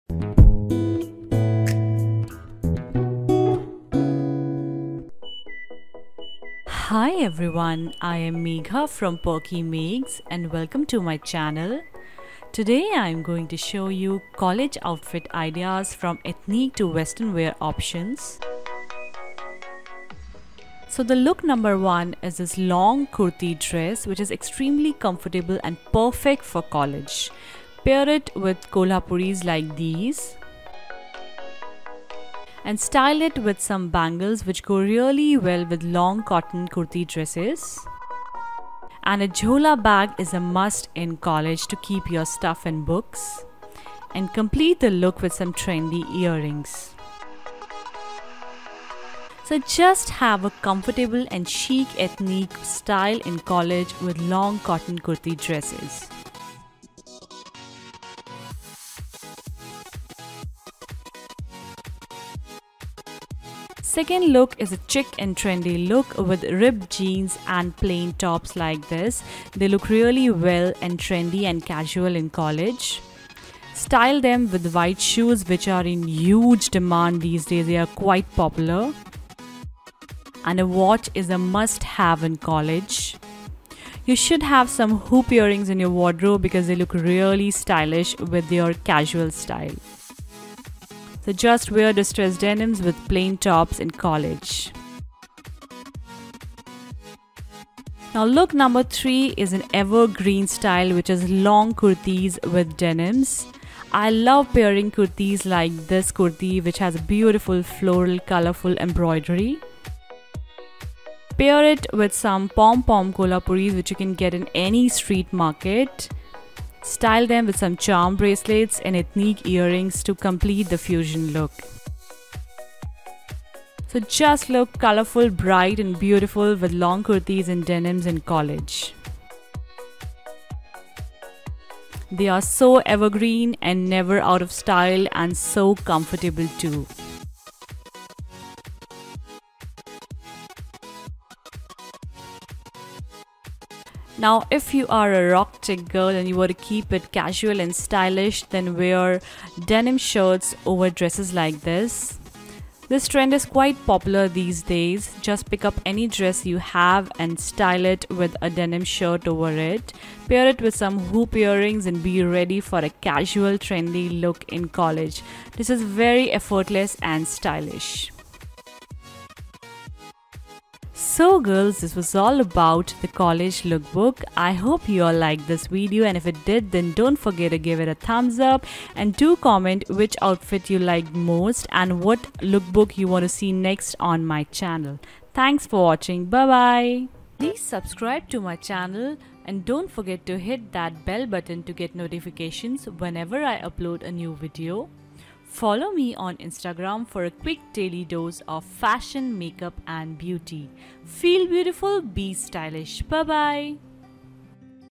College Outfit Ideas | Ethnic and Western Indian College Lookbook | Perkymegs
College outfit ideas with ethnic and western wear options. Indian college lookbook 2017.

Kolhapuris - Street shop

Look 2:

Top- H&M
Jeans- Diesel
Shoes- Koovs
Watch- French connection
Hoops- Forever 21

Look 3:

Kurti- Shree
Jeans- Diesel
pom pom chappals- Street shopping

Look 4:
Striped Dress- Gamiss.com
Jeans shirt- Elle
Shoes- Koovs
Hoops- Forever 21

Camera used

Lighting Setup used

Recording Mic